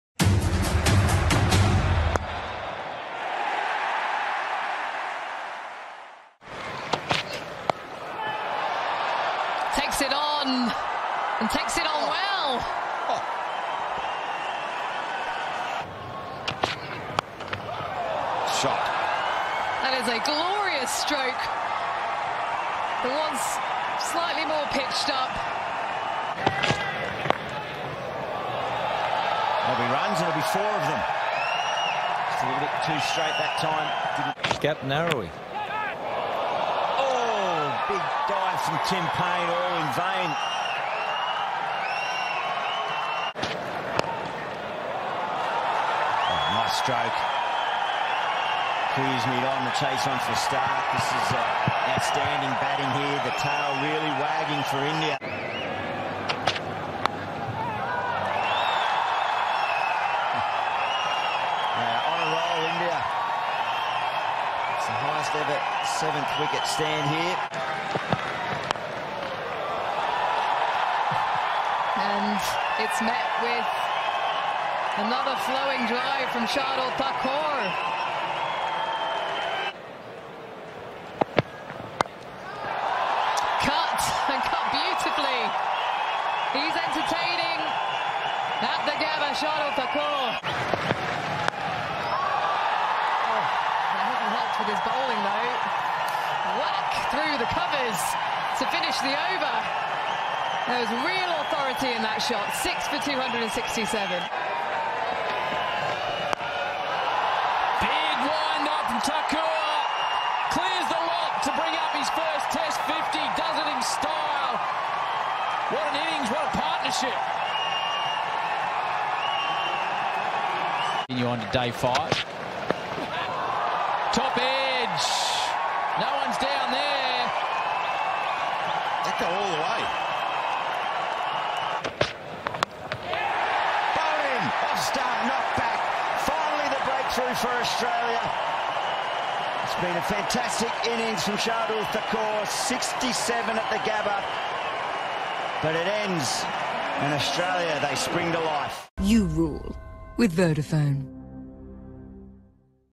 Shardul Thakur whacks Aussies around the Gabba | Vodafone Test Series 2020-21
A jaw-dropping counter-attack from India's No.8 as he smacked nine fours and two sixes in his knock of 67 for a maiden Test match fifty against Australia's vaunted bowling attack, including seeing off the second new ball.